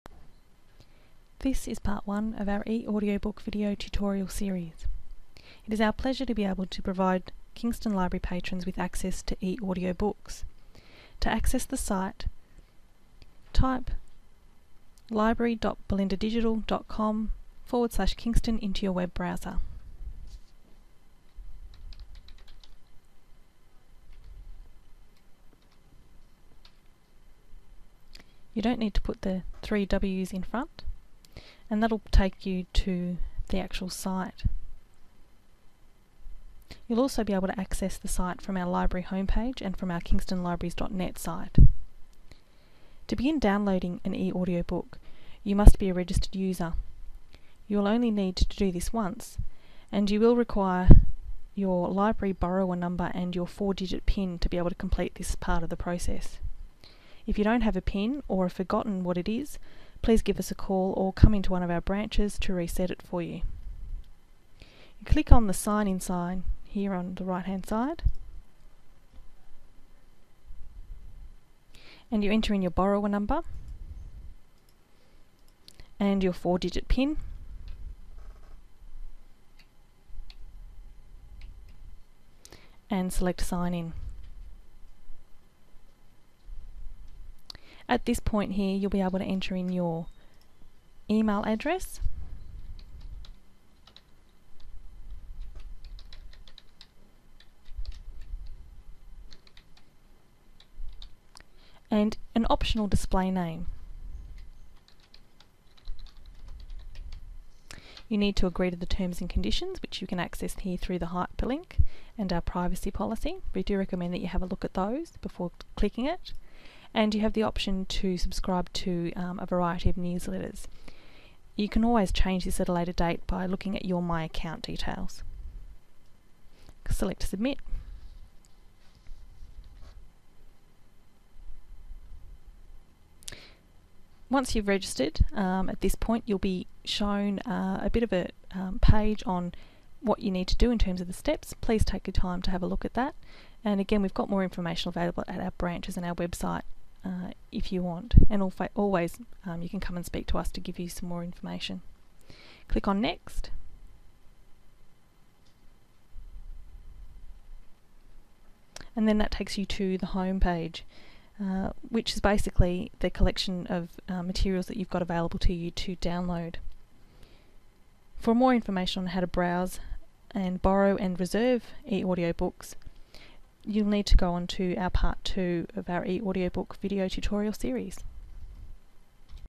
Kington Library - eAudio Book Downloads - Part 1 - How to Register
This is a short instructional video outlining how Kingston Library patrons can register to use the new eAudio book Download service. This video is part 1 in the series.